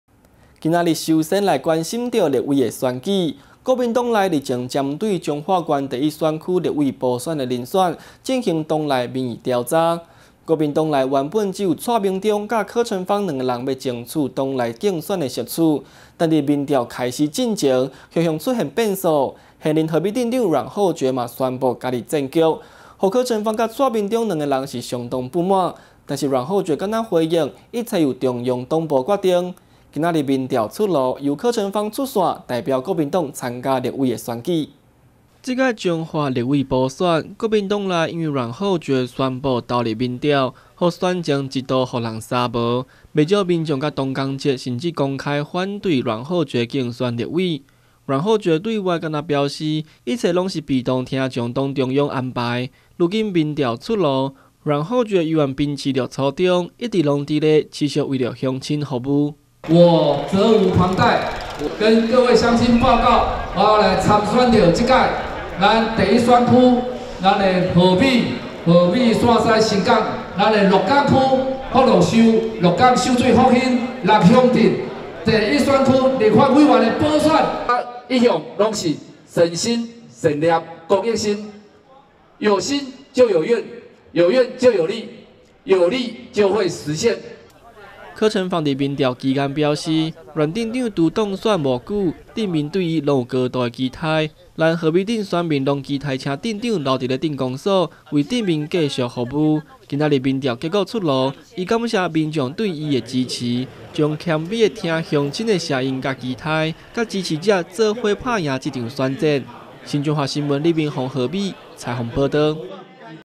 新彰化新聞20190110 彰化立委補選 國民黨黨內民調柯呈枋勝出
國民黨內日前針對彰化縣第一選區立委補選人選進行黨內民調，今天民調結果出爐，由柯呈枋取得領先，他將代表國民黨出馬競選立委席缺。國民黨內原由蔡明忠及柯呈枋兩人角逐黨內競選席次，但民調開始前突然出現變數，現任和美鎮長阮厚爵也宣布投入民調角逐，讓柯蔡兩人相當不滿，但阮厚爵僅回應一切由中央黨部決定，今民調出爐，由柯呈枋出馬角逐立委席缺。